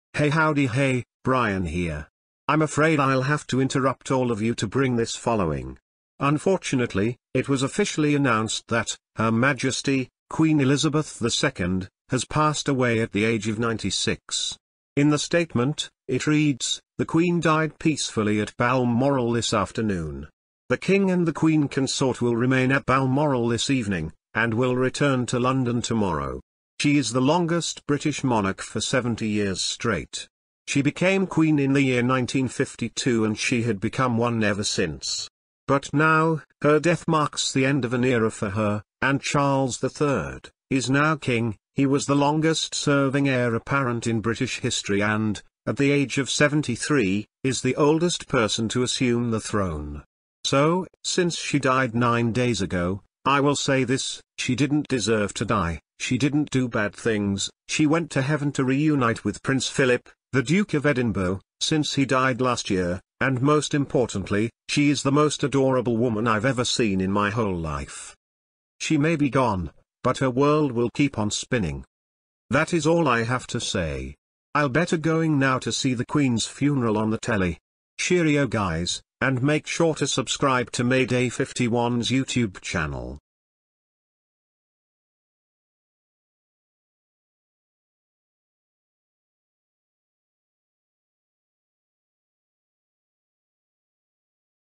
Brian the Bunny Rabbit's Announcement on Queen Elizabeth II's Death
"Brian the Bunny Rabbit has to interrupt them for the following announcement about Queen Elizabeth II dying at age 96.

This is for @BrianBBunny who created this character back in 2016.

R.I.P. Queen Elizabeth II (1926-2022)